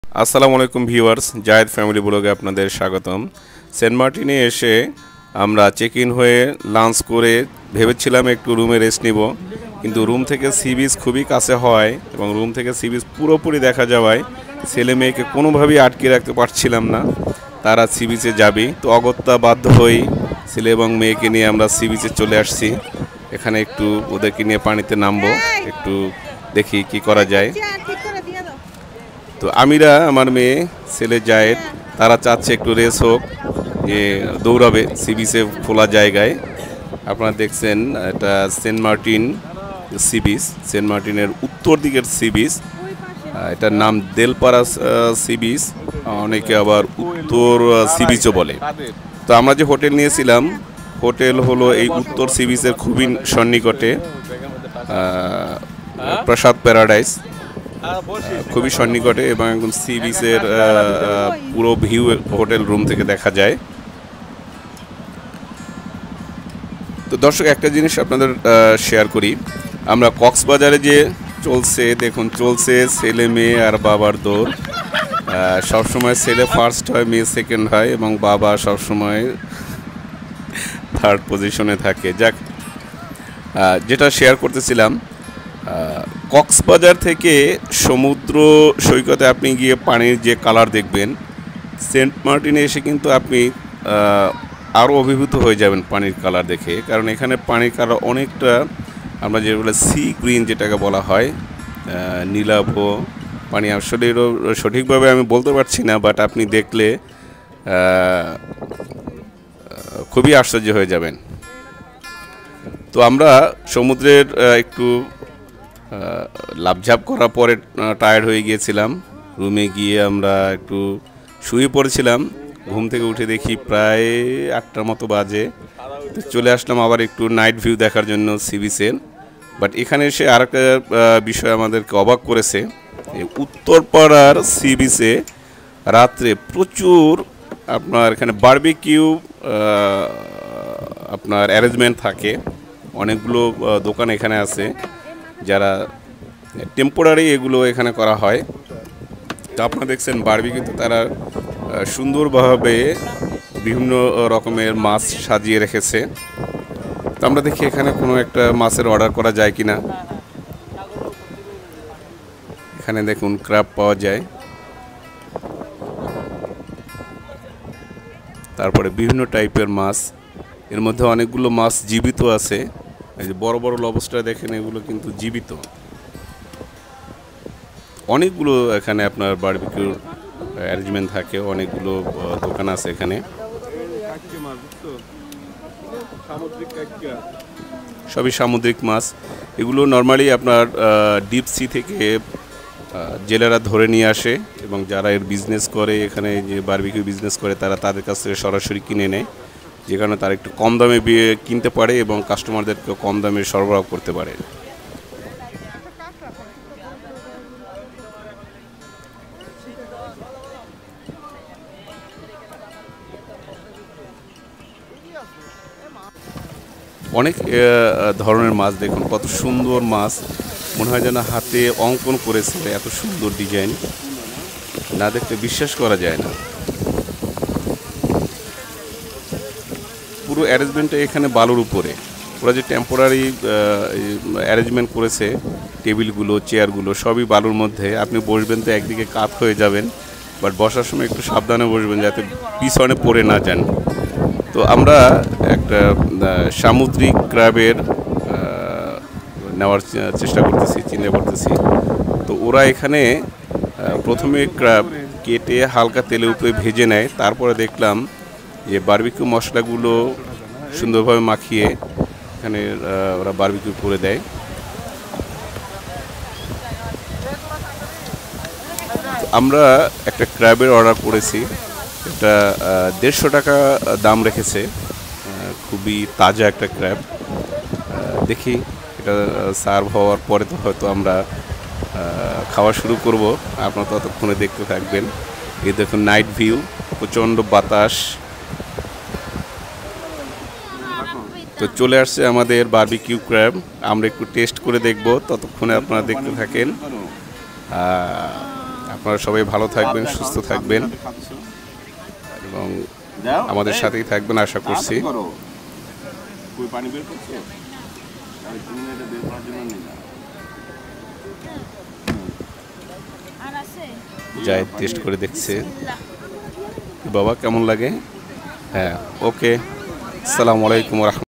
Beach BBQ in Saint Martin// vlog-17
Hotel prasad Paradise amra stay korasi.. room rent-6000tk per night..